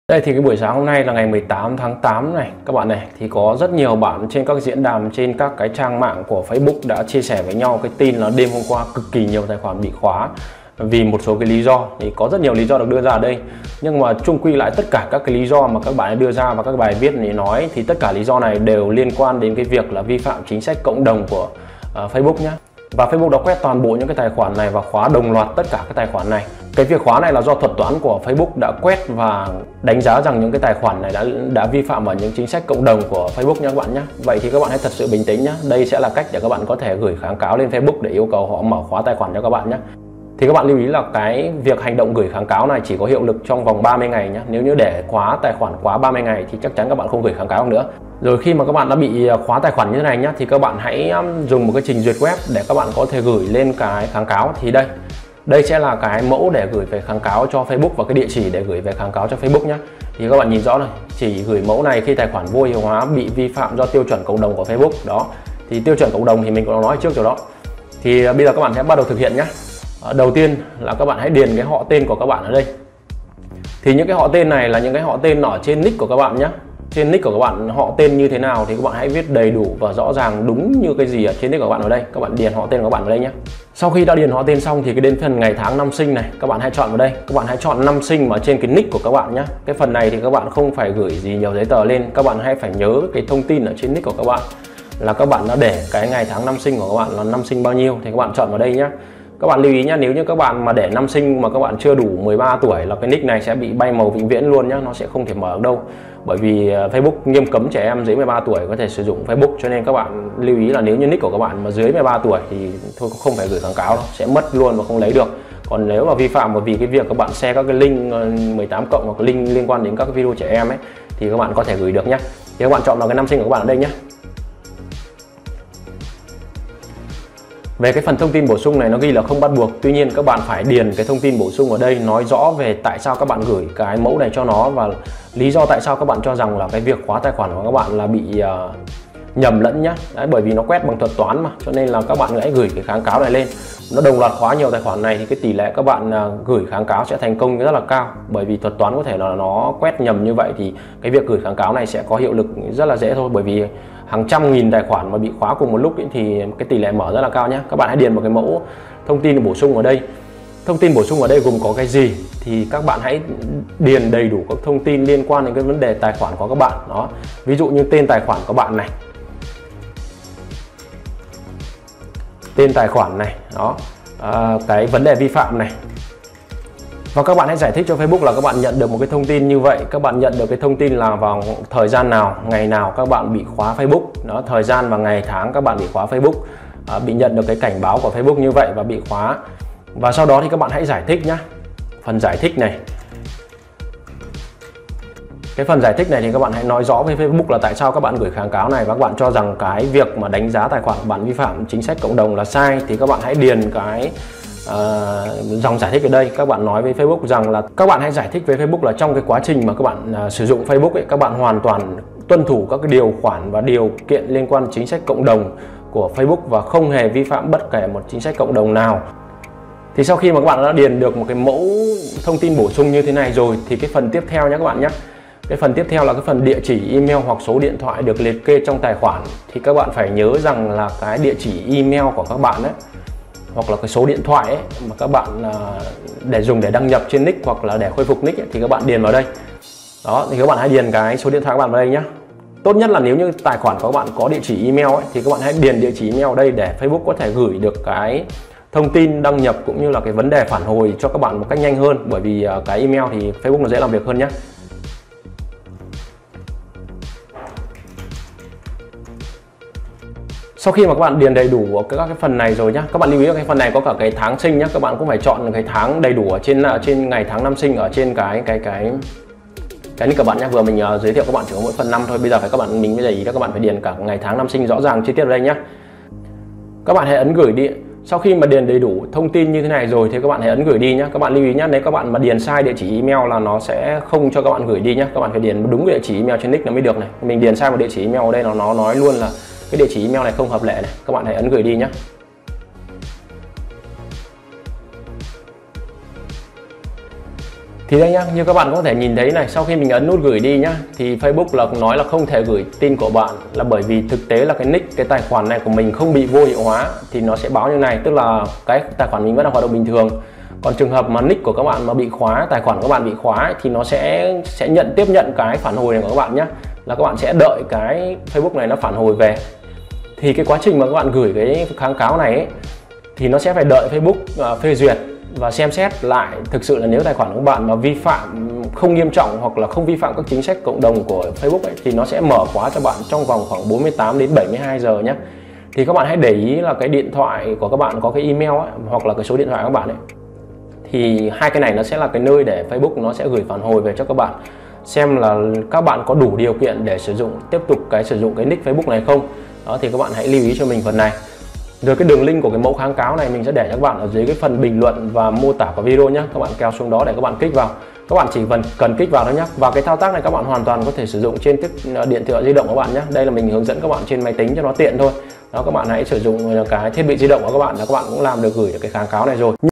Cách Lấy Lại Tài Khoản Facebook Bị Vô Hiệu Hoá . Facebook Bị Khoá Hàng Loạt 18/08/2021 (mới)
Hiện tại hàng loạt các tài khoản facebook bị vô hiệu hoá vì lí do liên quan đến vi phạm chính sách cộng đồng của facebook. làm thế nào để có thể lấy lại tài khoản facebook bị vô hiệu hoá như thế này. Theo mình tìm hiểu thì đa phần các tài khoản facebook bị khoá hàng loạt này là do các tài khoản này vi phạm vào chính sách cộng đồng chia sẻ các đường link nhậy cảm và các đường link liên quan đến trẻ em nên tài khoản facebook của bạn bị vô hiệu hoá. Hãy thật bình tĩnh và làm theo hướng dẫn của mình để mở khoá tài khoản facebook bị vô hiệu hoá của bạn nhé .